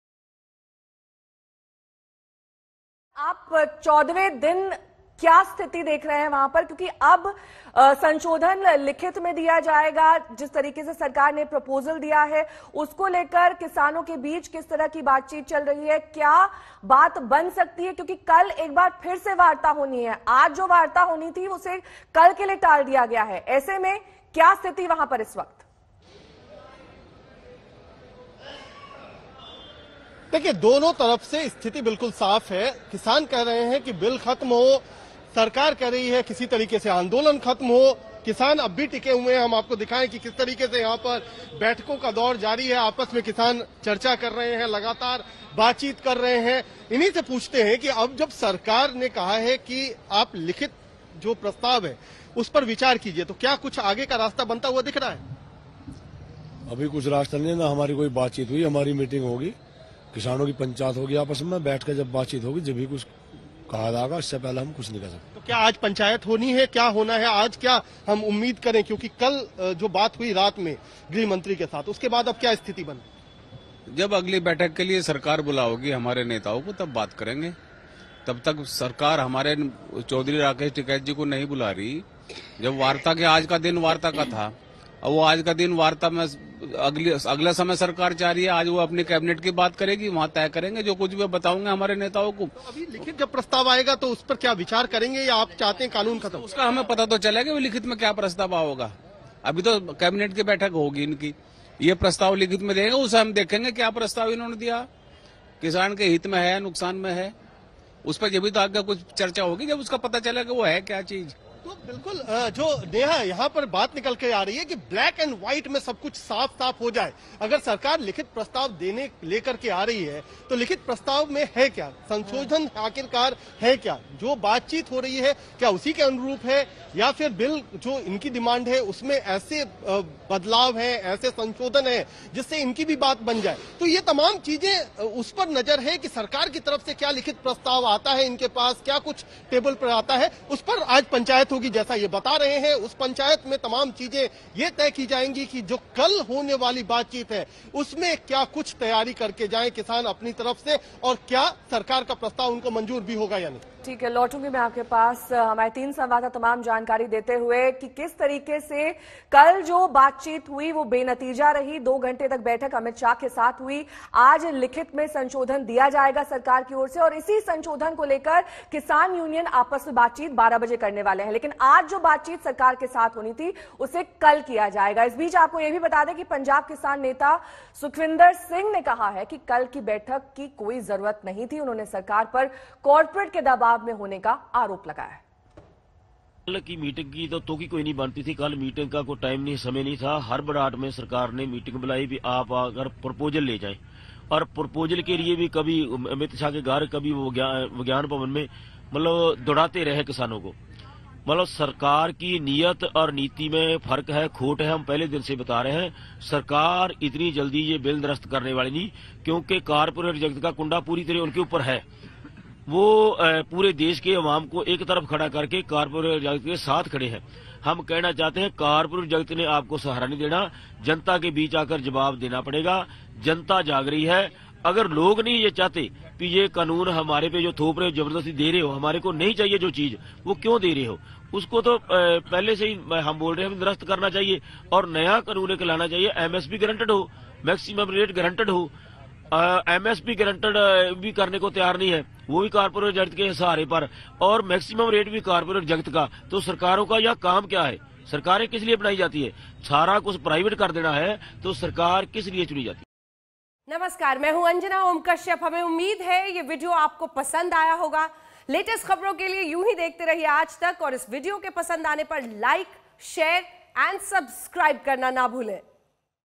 Farmers Protest: किसान आंदोलन का 14वां दिन, अब तक नहीं बनी बात!
Farmers Protest: किसानों का आंदोलन खत्म होने का नाम नहीं ले रहा है. बीती रात amit shah और किसाों की 2 घंटे बैठक भी हुई. लेकिन उसमें भी बात नहीं बन पाई.